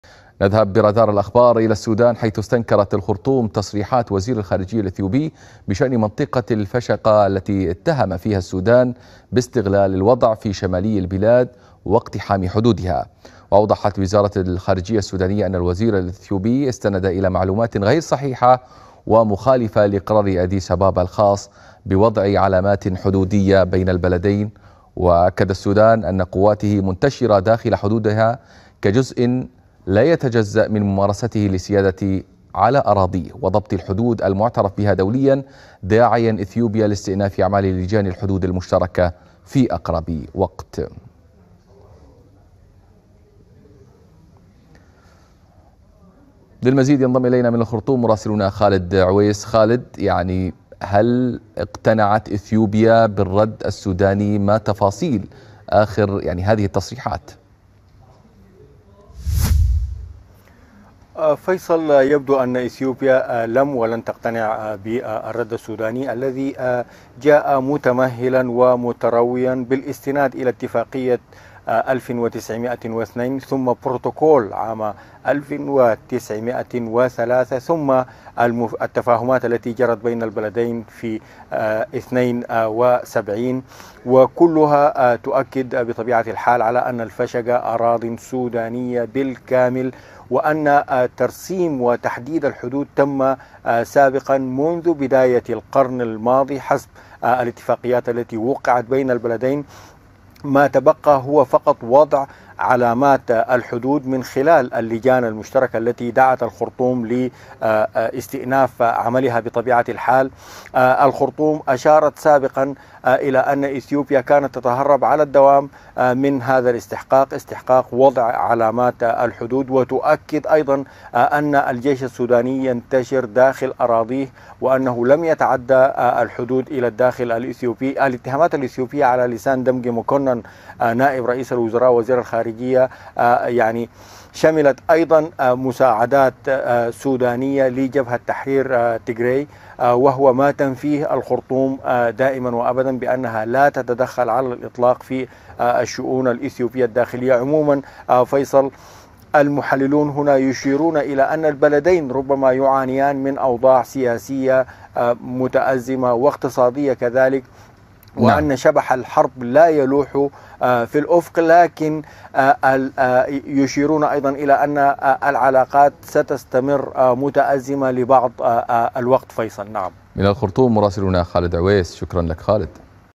الخرطوم تستنكر تصريحات إثيوبيا بشأن منطقة "الفشقة"
استنكر السودان تصريحاتِ وزير الخارجية الإثيوبي بشأن منطقة الفشقة التي اتهم فيها الخرطوم باستغلال الوضع في شمالي بلاده واقتحام حدودها.وأوضحت وزارة الخارجية السودانية أن الوزير الإثيوبي استند إلى معلوماتٍ غيرِ صحيحة ومخالفة لإقرار إثيوبيا الخاص بوضع علامات حدودية بين البلدين
وأكد السودان أن قواتِه منتشرةٌ داخل حدودها كجزءٍ لا يتجزأ من ممارستِه لسيادته على أراضيه وضبط الحدود المُعترَفِ بها دولياً، داعيا إثيوبيا لاستئناف أعمال لجان الحدود المشتركة في أقرب وقت.